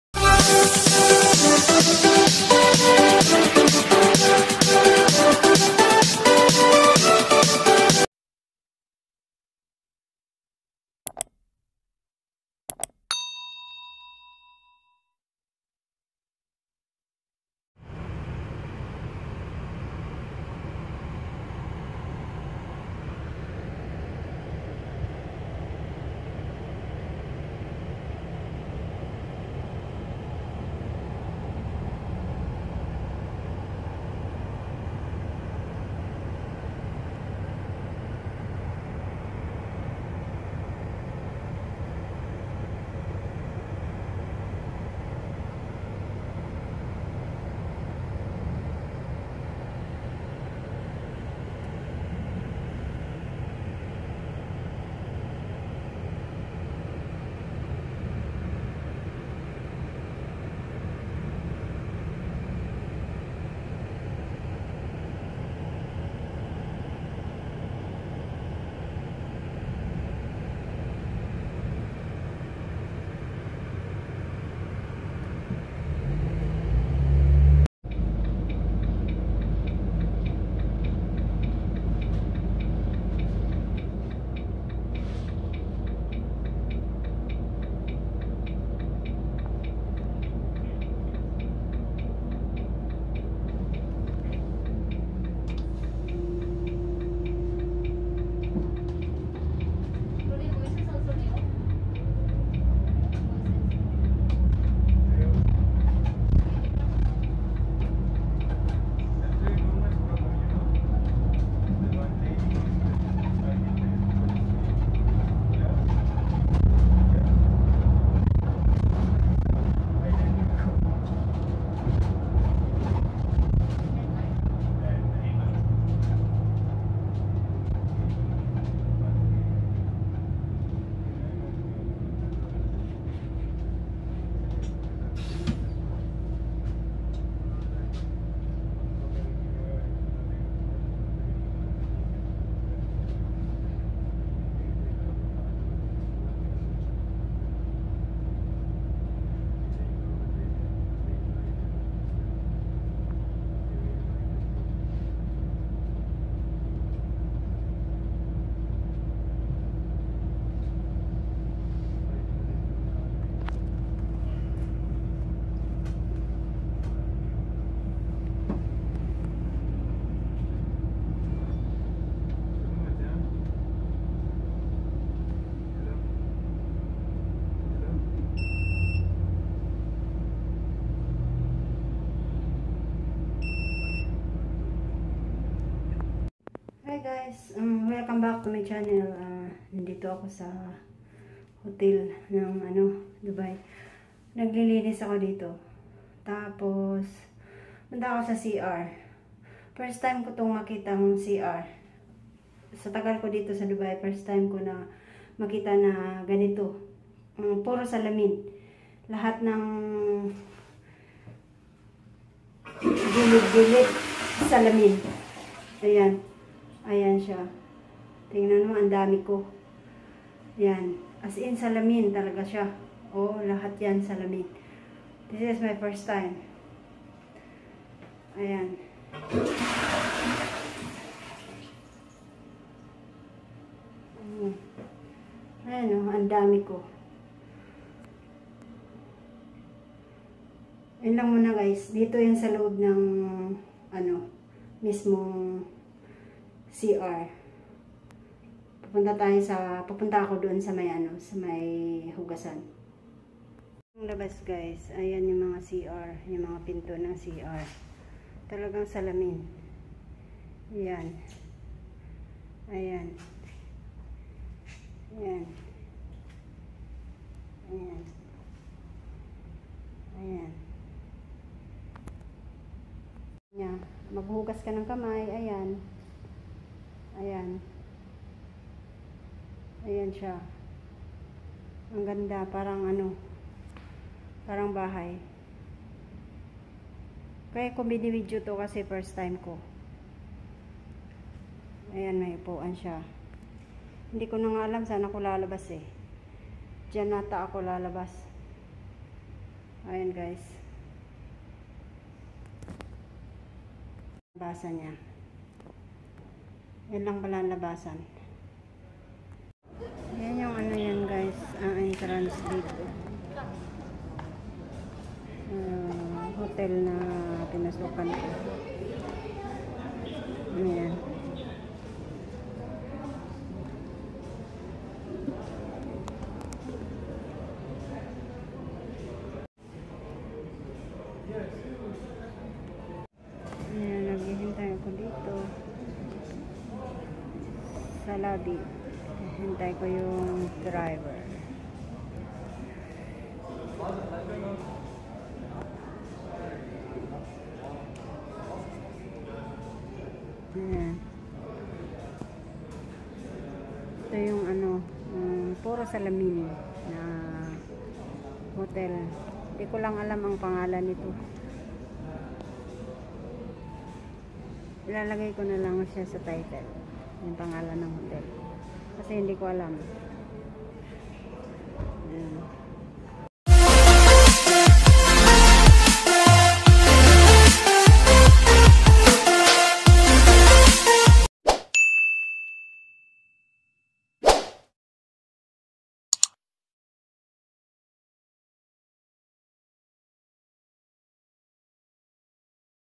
My Video Compilation Part1 (arlene arcebal CHANNE)
This is My First Time I see here in Dubai the Toilet is all glass